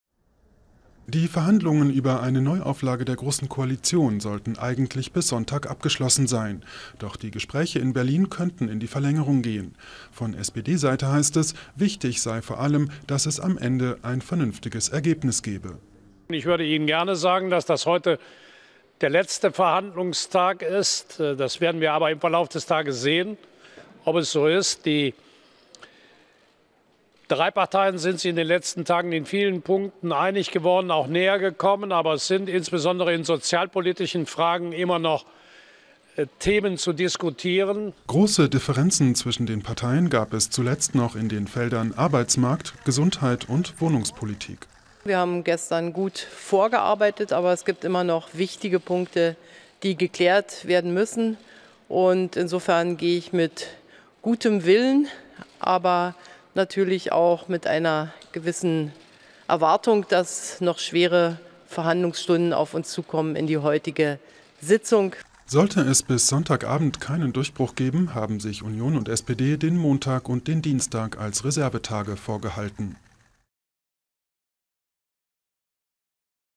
Groko-Verhandlungen: Union und SPD sind sich noch nicht einig
Die Verhandlungen über eine Neuauflage der großen Koalition sollten eigentlich bis Sonntag abgeschlossen sein. Große Differenzen zwischen den Parteien gab es zuletzt noch in den Feldern Arbeitsmarkt, Gesundheit und Wohnungspolitik.